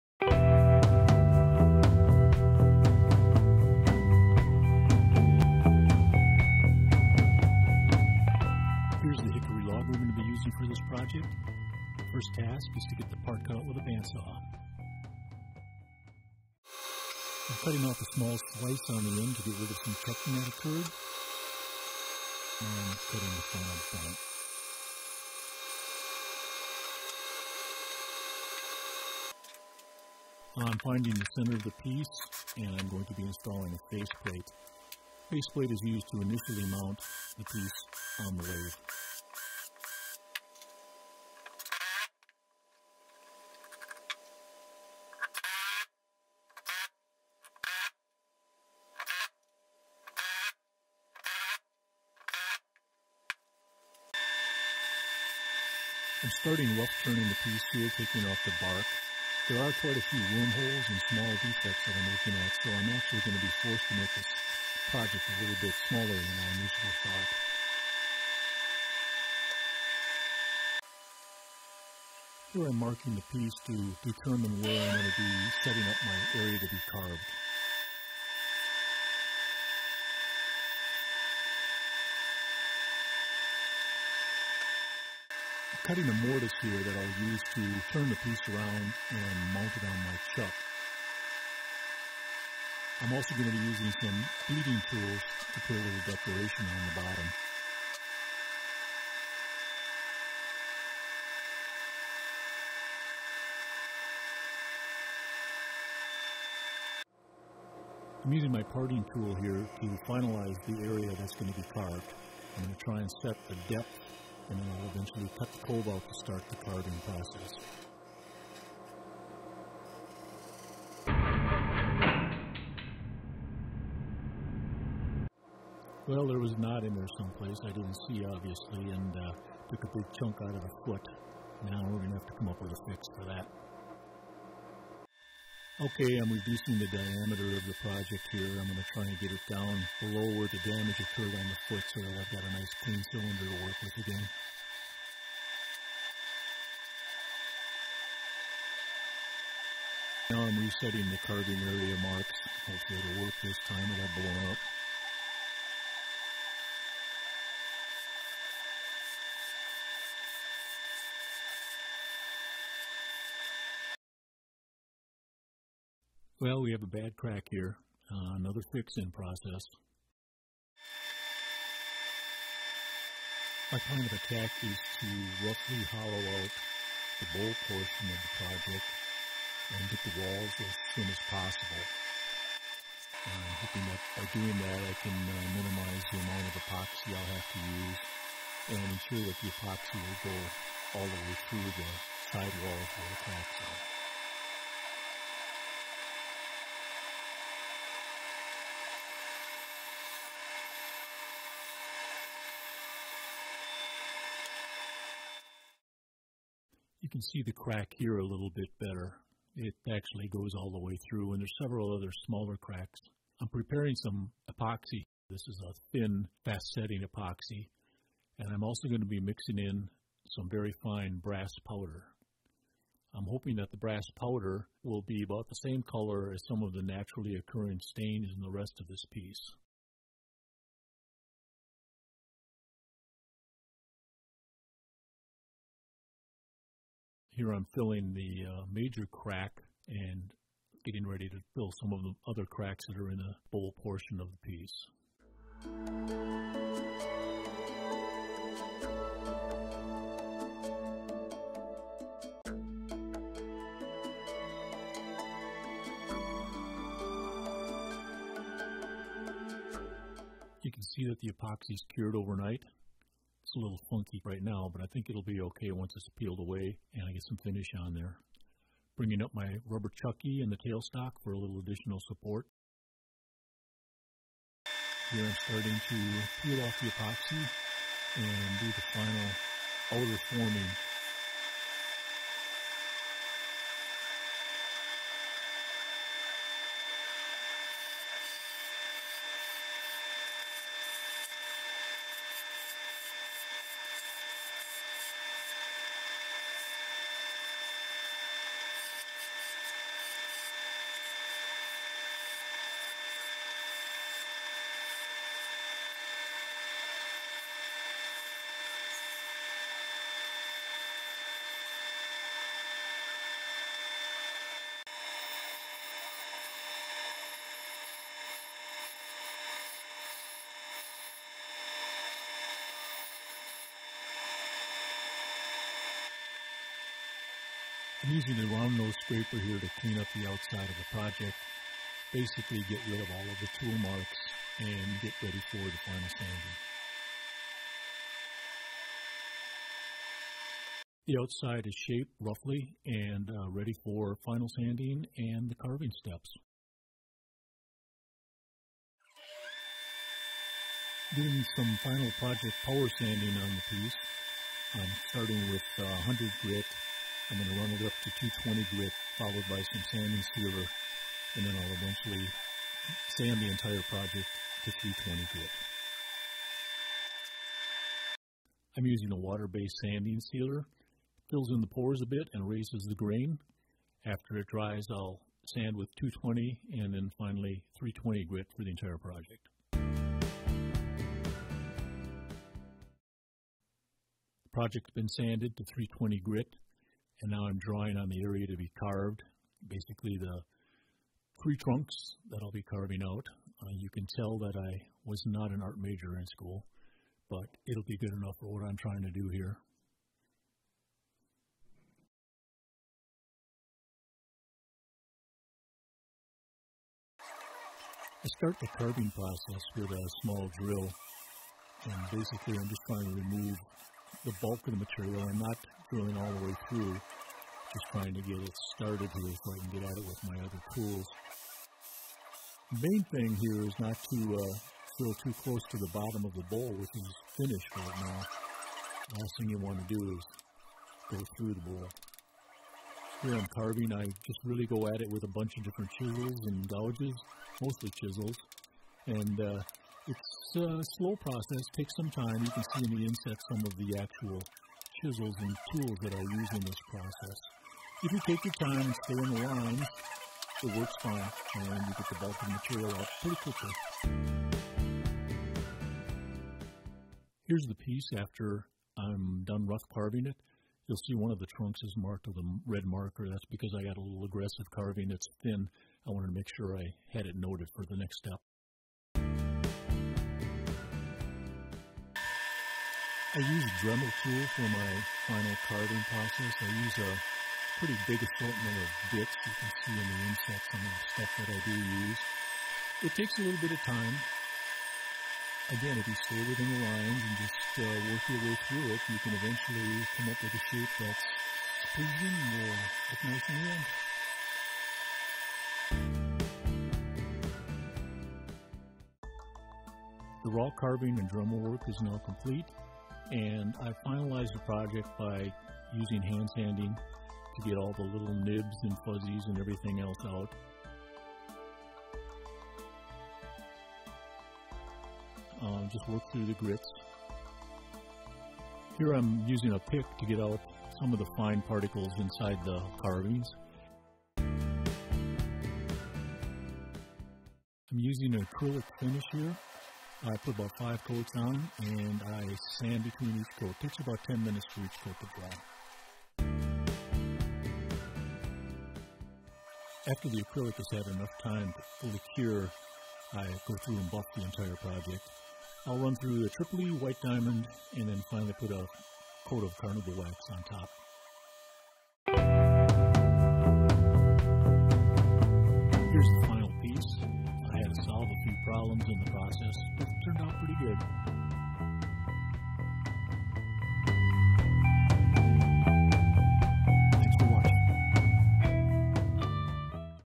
Turned-Carved Hickory Bowl Build
Creating an elevated, turned, hickory bowl with carved legs.